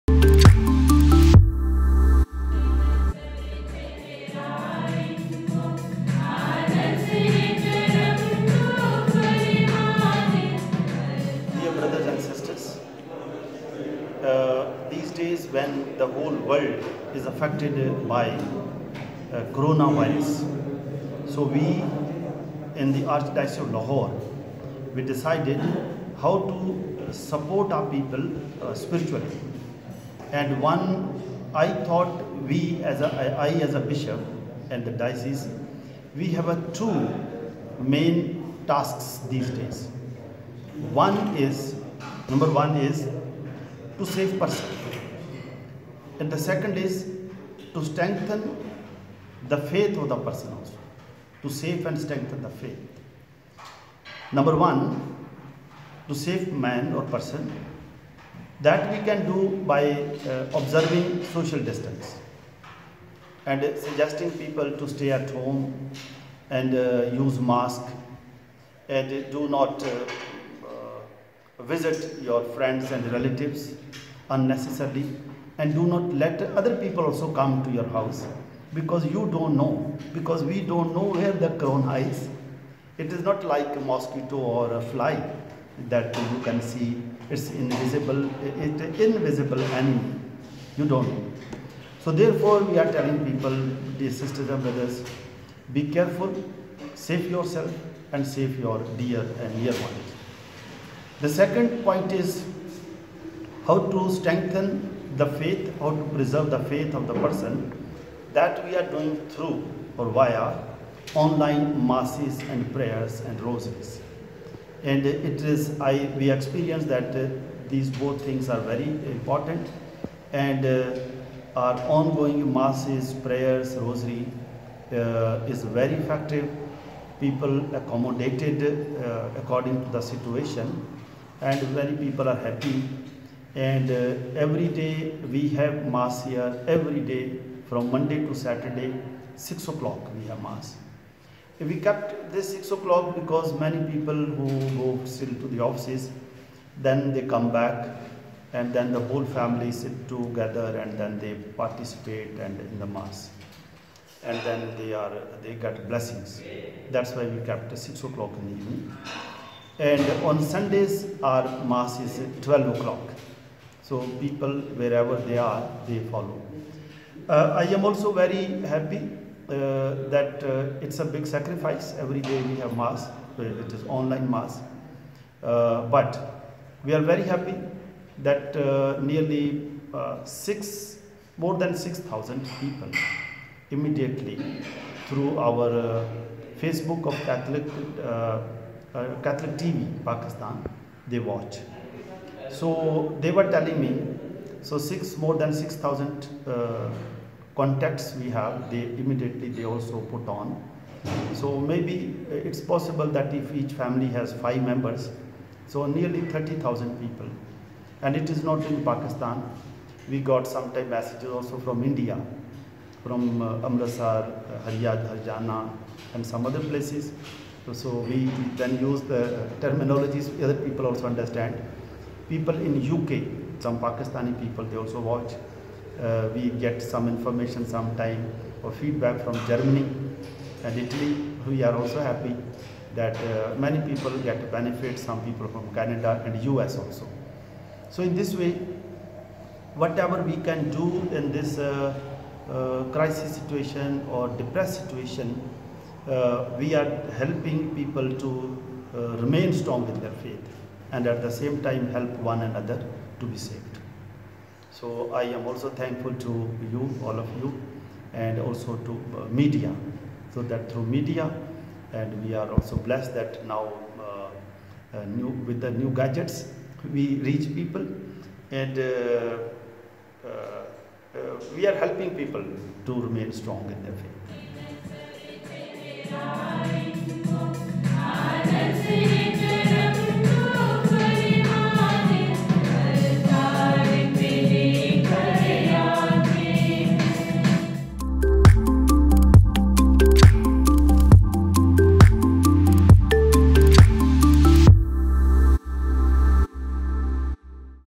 Pakistan Catholics keep the faith online amid Covid 19 pandemic
Archbishop Sebastian Shaw is providing spiritual support to the faithful amid virus lockdown by hosting daily online masses in evening on weekdays and in afternoon on Sunday. More than 6,000 viewers from Pakistan and abroad watch the prayers live on Facebook page of Catholic TV. A small congregation of 20 including nuns, catechists and a few faithful attend the mass at a makeshift studio in bishop’s house.  Our main task is to save lives and strengthen faith, says Archbishop Shaw who is also spearheading “Make Masks and Save Lives” campaign.

Want to read the full story? Here is the link

Follow UCANews on the following handles
Twitter.com/@ucanews
Instagram.com/uca.news
Facebook.com/ucanews